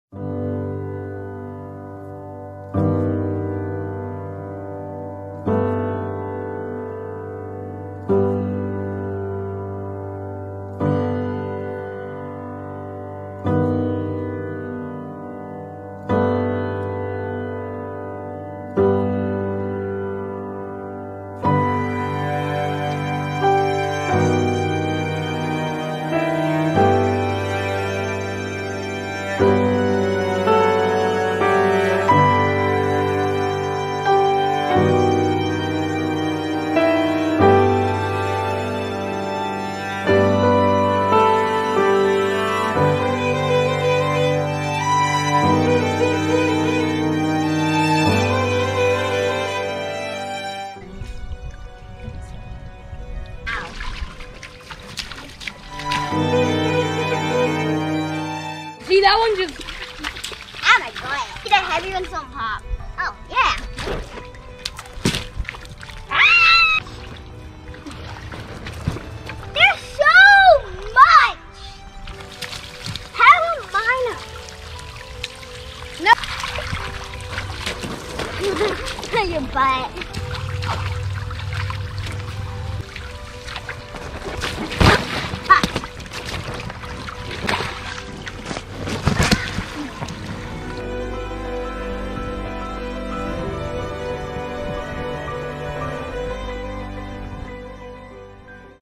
Chalk Art & Water Slide Out Back Liam & Laken - Lewes, DE - 7/5/2020
The boys visited Great Grandmom before family chalk fun and water slide out back at TheWrightPlace